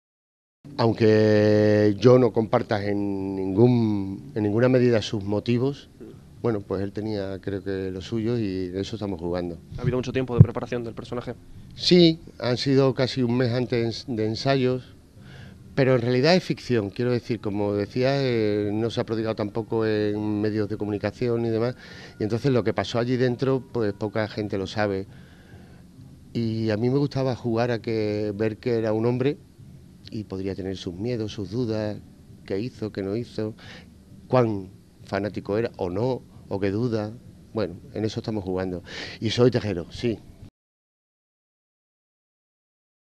Paco Tous se mete en la piel de Tejero en el cine
Precisamente hoy, 23 de febrero, se estrena una película protagonizada por Paco Tous en el papel de Tejero, que nos invita a realizar un viaje en el tiempo hasta 1981. El protagonista insiste en que es ficción, ya que se adentra en los pensamientos y los miedos del Teniente Coronel de la Guardia Civil.